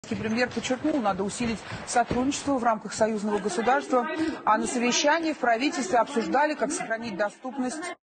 В ефірі російського Першого каналу з'явилася дівчина із плакатом "Ні війні!"
В эфире российского Первого канала появилась девушка с плакатом "Нет войне!"
On the air of the Russian First Channel, a girl appeared with a poster "No to war!"